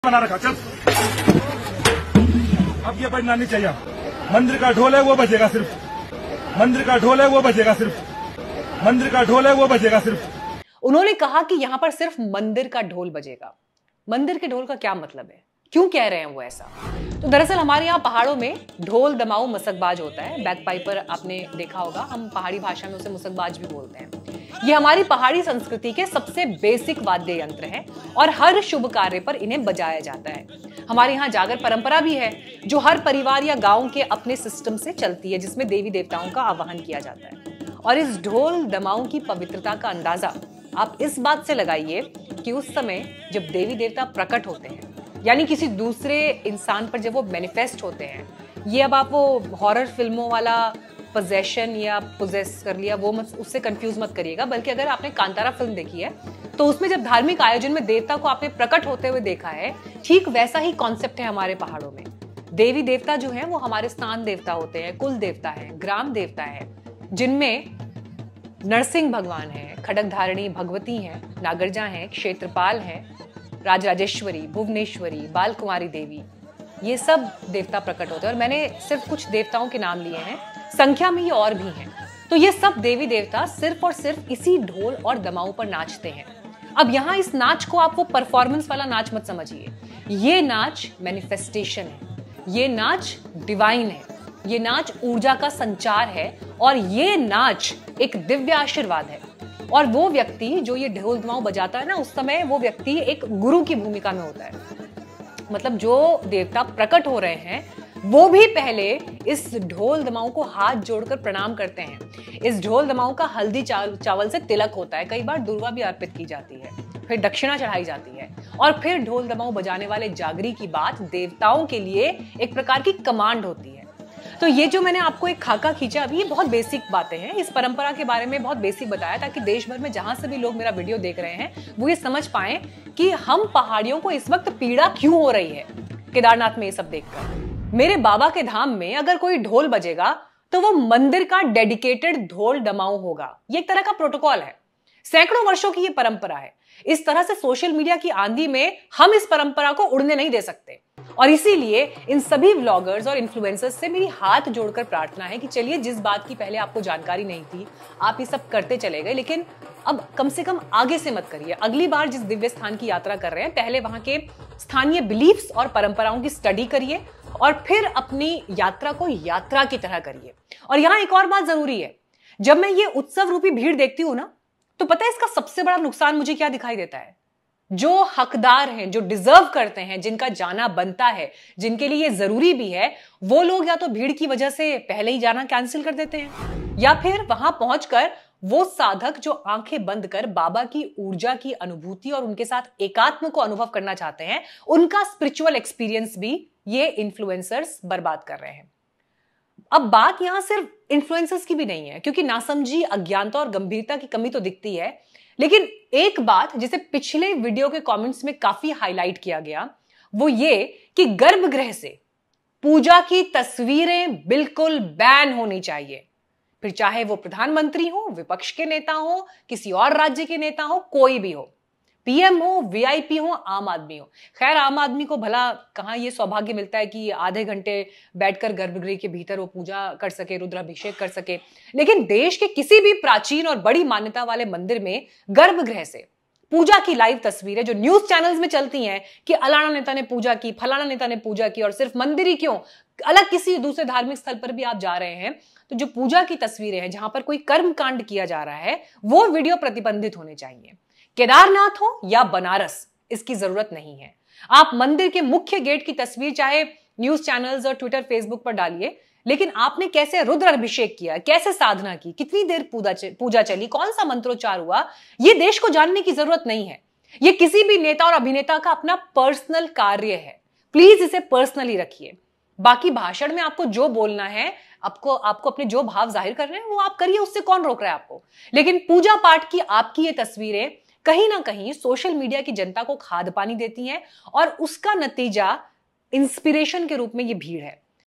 Kedarnath Yatra | Chardham Yatra 2024 | Importance Of Dharma & Culture in Temple | Uttarakhand News
इस वीडियो में उत्तराखंड के केदारनाथ में आकर धोल नगाड़े बजाकर शोर मचाने वालों के लिए प्रमुख संदेश। पहाड़ के कल्चर और धर्म के आधार पर दर्शन लाभ कैसे उठाया जायें। डिटेल वीडियो में।

KEDARNATH DHAM YATRA 2024 CROWD | UTTARAKHAND CHAR DHAM YATRA | Lord Shiv | Reels In Kedarnath